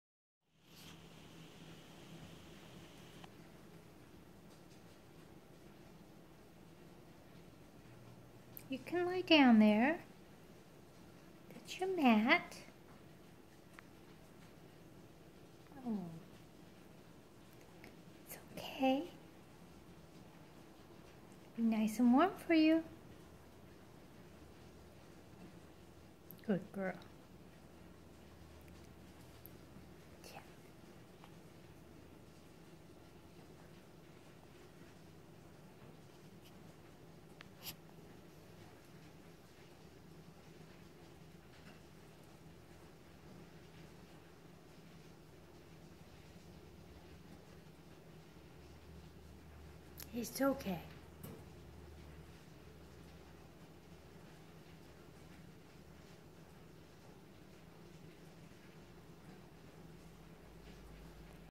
Mindy the Toy Manchester Terrier
Chilly evening in Georgia and Mindy the Toy Manchester Terrier just wants to be warm!